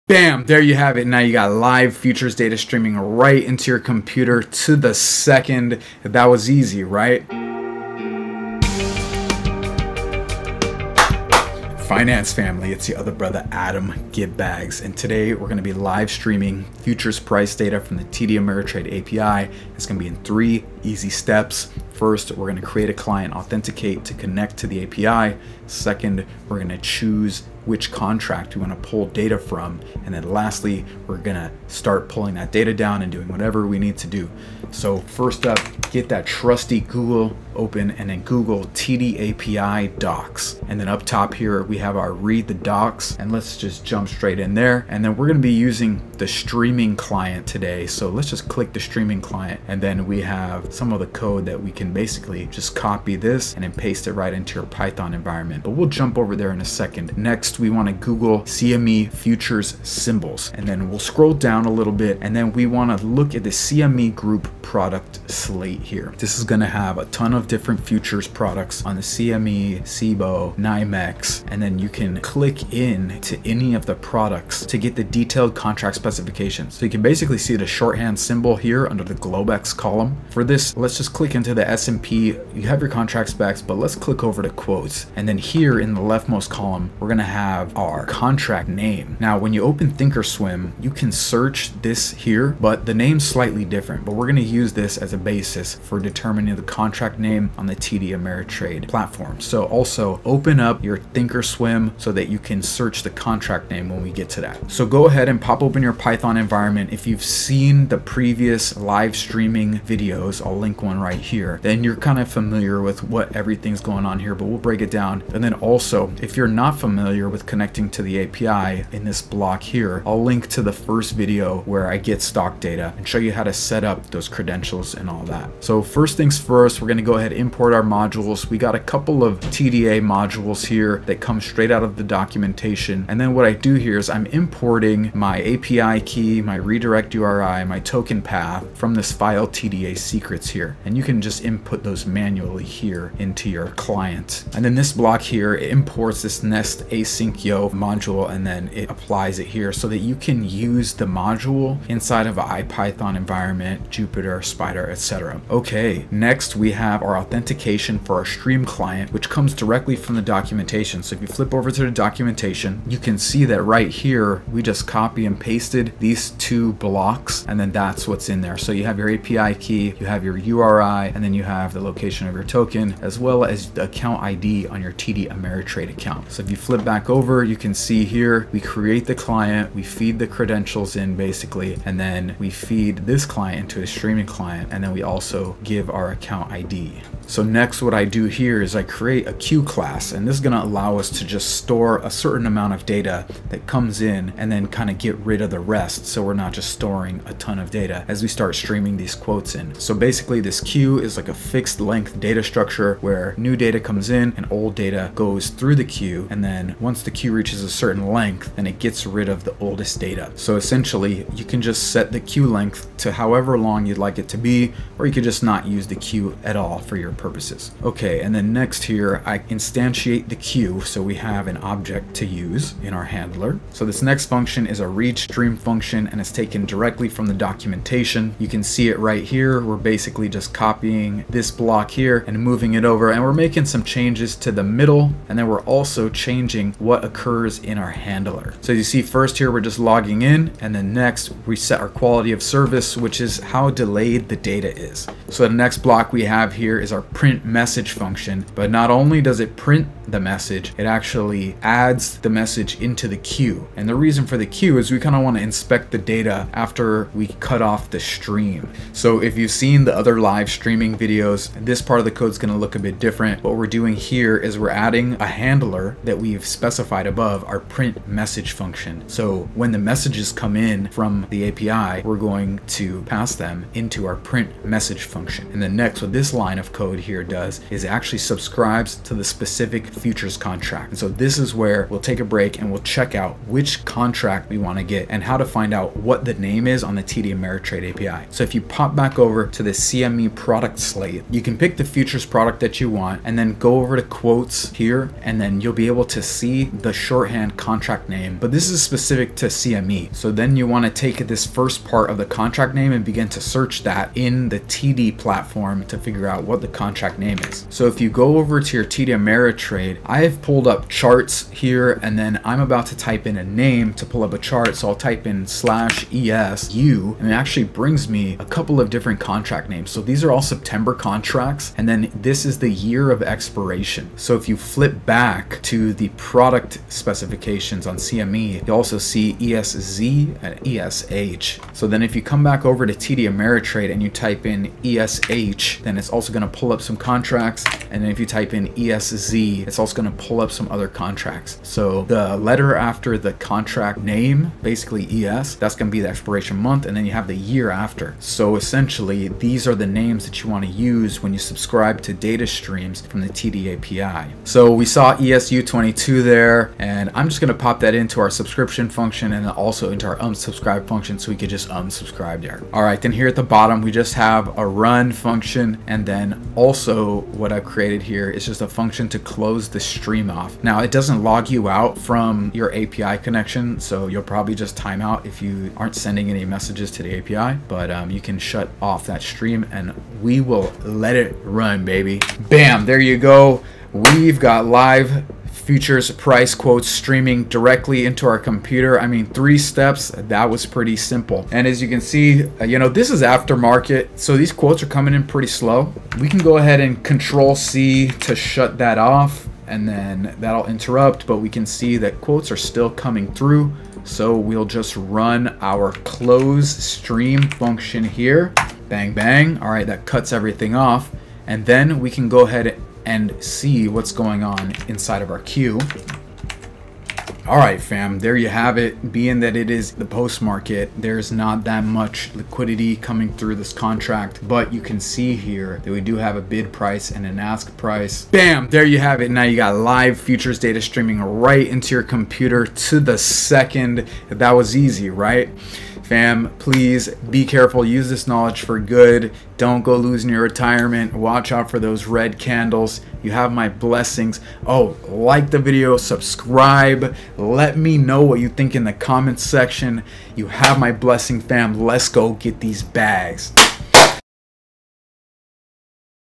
Real Time Streaming Futures Price Data With TD Ameritrade API In Python
Let's take a look at how to stream real time futures price data for exchange traded futures contracts using the TD Ameritrade API in Python.

0:37 open the documentation, futures product list, and TD ThinkOrSwim (links below!)
2:06 import modules
2:35 authenticate streaming client
3:07 create a queue class to store data from stream
3:43 overview of read_stream() function
4:04 defining a "handler" function to handle stream messages
4:18 adding a handler
4:34 subscribe to futures product stream
4:40 get futures contract name to use when subscribing to stream
5:53 add futures contract name to sub/unsubscribe functions
6:01 review unsubscribe function
6:18 subscribe to the data stream
6:39 unsubscribe from the data stream
6:50 inspect the data in the queue

There are a bit of specifics in choosing the futures contract name when streaming futures price data, so be sure to specify the exact contract name you are looking for. It's fairly straight forward to find the contract name using the CME products list and the search function in ThinkOrSwim.

The set up of the code is similar to the equity/option real time streaming with the TD Ameritrade API; however, in the body of the read_stream function, you just have to swap in StreamClient.level_one_futures_subs(...) and StreamClient.add_level_one_futures_handler(...)

Easy business, finance family.